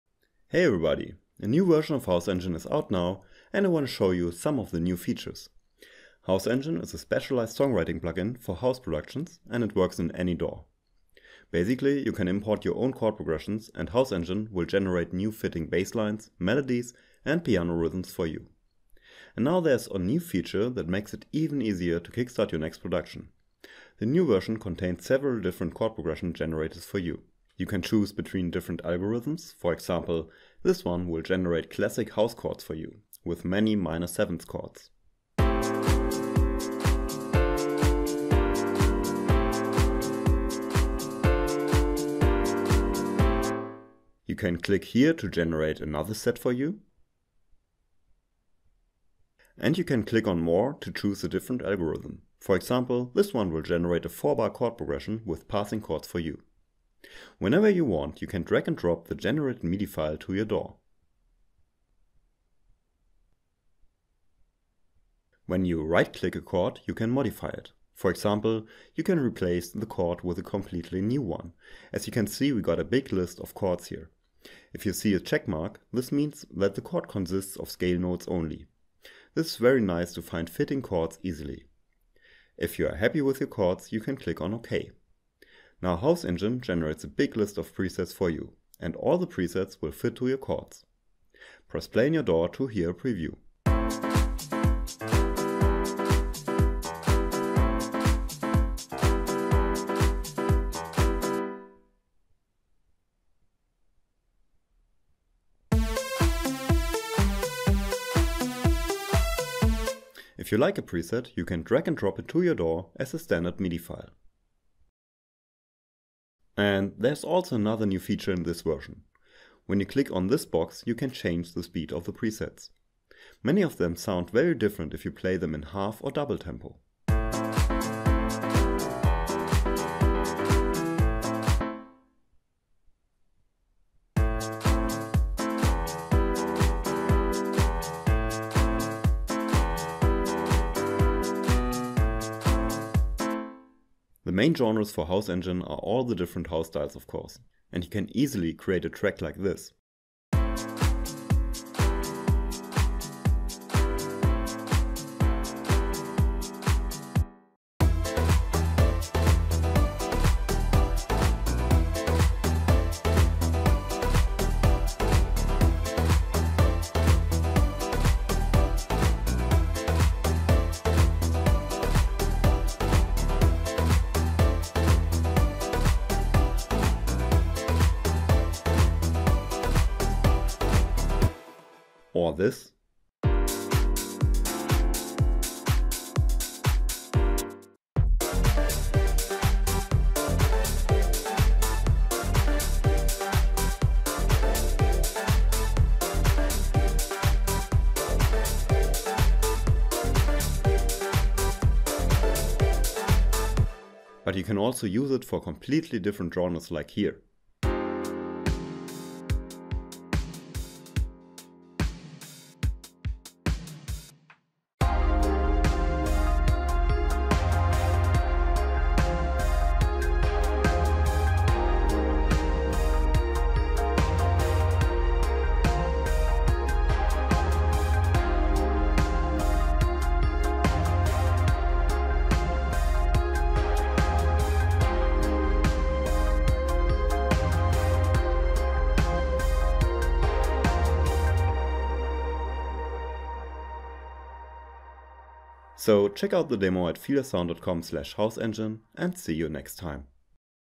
How to create House chords and melodies automatically with HouseEngine
♥ Join the FeelYourSound newsletter to get a subscriber discount on HouseEngine and other songwriting products